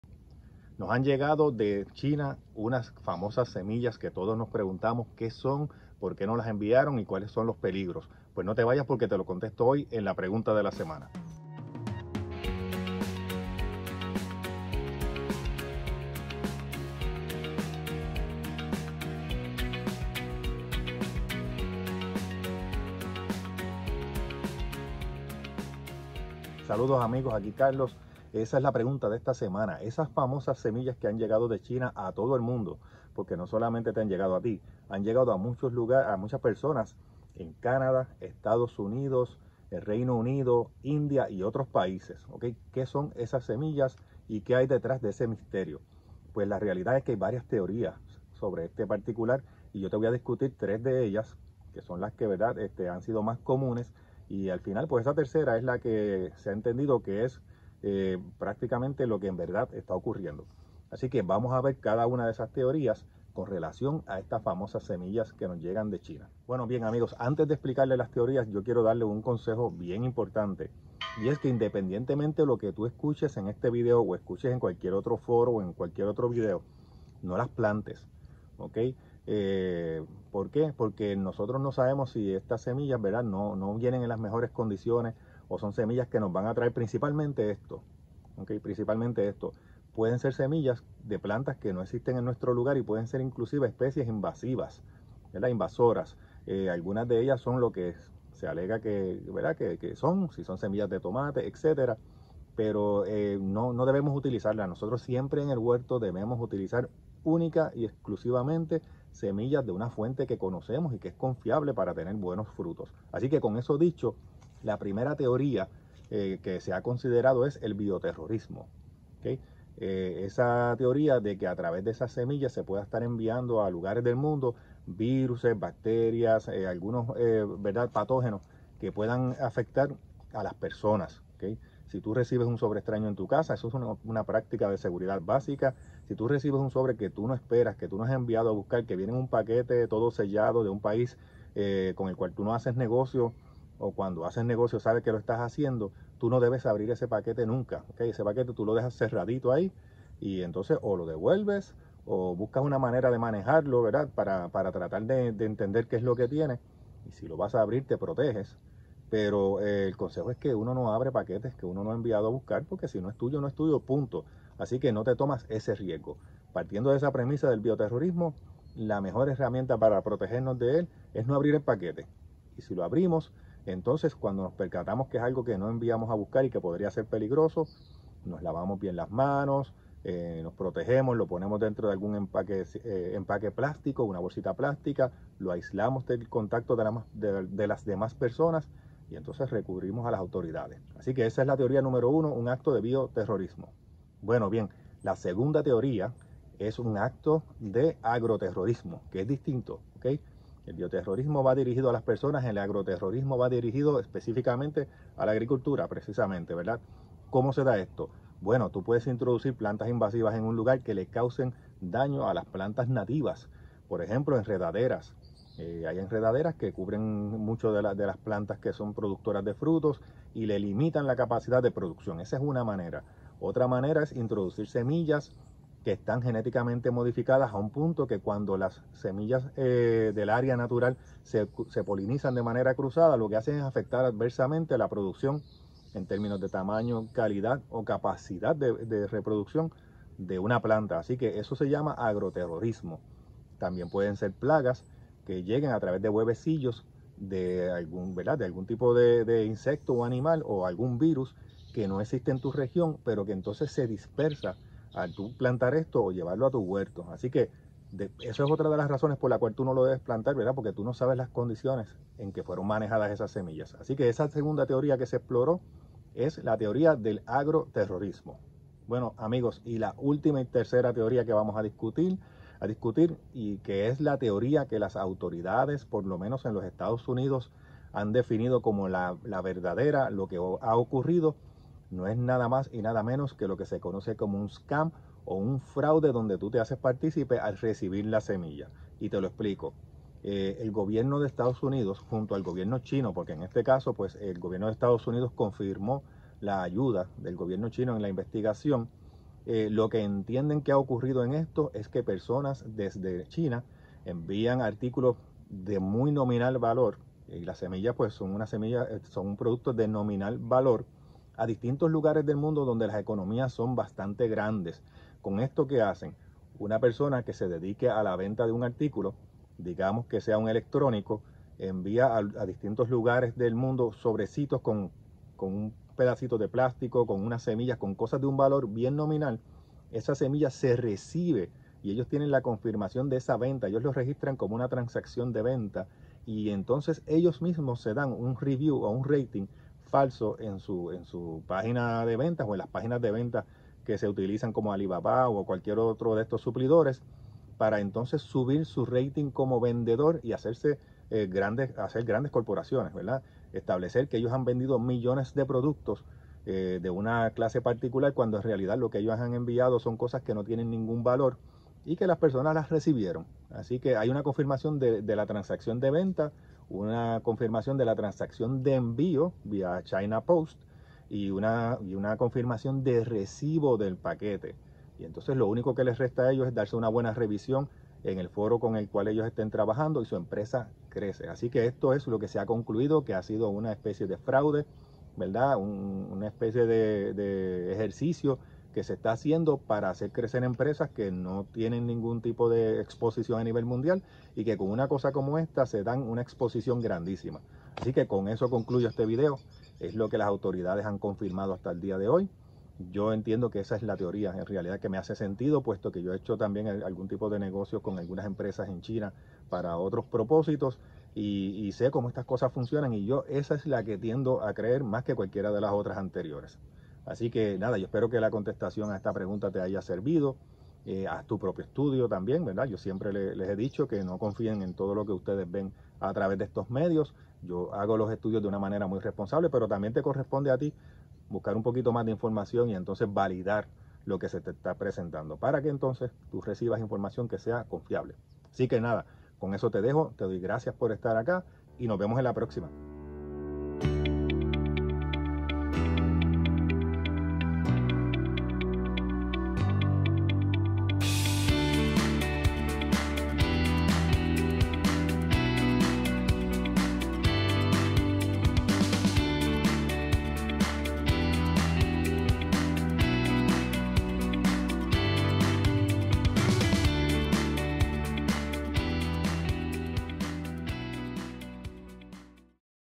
Semillas Chinas; ¿Qué Son? (Preguntas de la Semana #19)
Te explicamos cómo debes manejar el asunto si te llegan las famosas “semillas chinas”

Video sobre envío de semillas

Enlace al USDA
 USDA APHIS | USDA Investigates Packages of Unsolicited Seeds from China